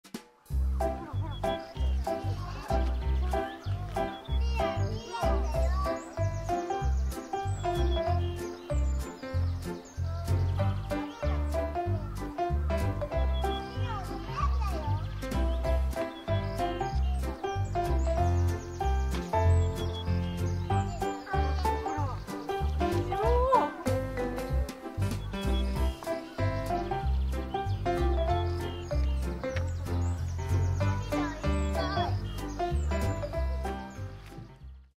イケメンゴリラのシャバーニ😊㊗️
2024年4月12日 金曜日 10:48

名古屋市にある東山動植物園で撮影したイケメンゴリラとして有名なシャバーニです。

青白く輝いて美しいと思います。

Friday, April 12, 2024 10:48

This is Shabani, a famous handsome gorilla, photographed at Higashiyama Zoo and Botanical Gardens in Nagoya City.

I think he's beautiful with his pale blue glow.